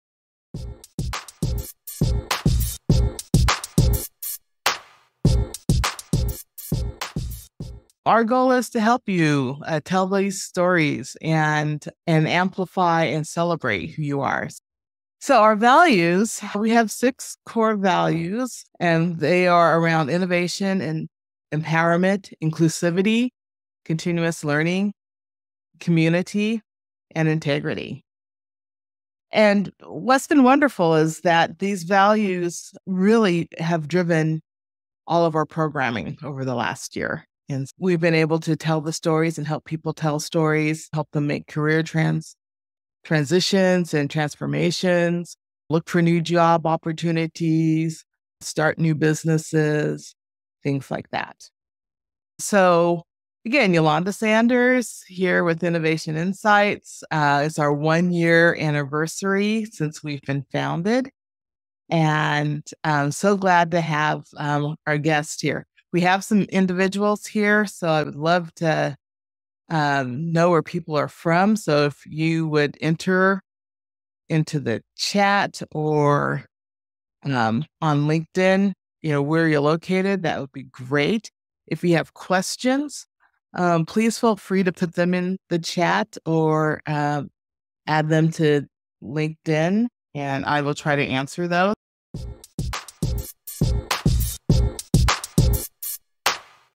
Our Core Values are to Amplify and Celebrate Who You Are
Join Dr. Eulanda Sanders as she reflects on INNOVATION Insights' first anniversary. Highlighting six core values—Innovation, Empowerment, Inclusivity, Continuous Learning, Community, and Integrity—Eulanda shares how these principles have shaped their programming and helped individuals tell their stories, transition in their careers, and embark on new business ventures. Engage with Eulanda and the community by sharing your location and questions on LinkedIn or in the chat.

00:00 Introduction and Core Values
00:29 Impact and Achievements
00:49 Anniversary Celebration
00:57 Audience Interaction
01:13 Q&A Session

Discover exclusive gift items, empowering gift cards, transformative seminars and workshops, and free creative coloring pages.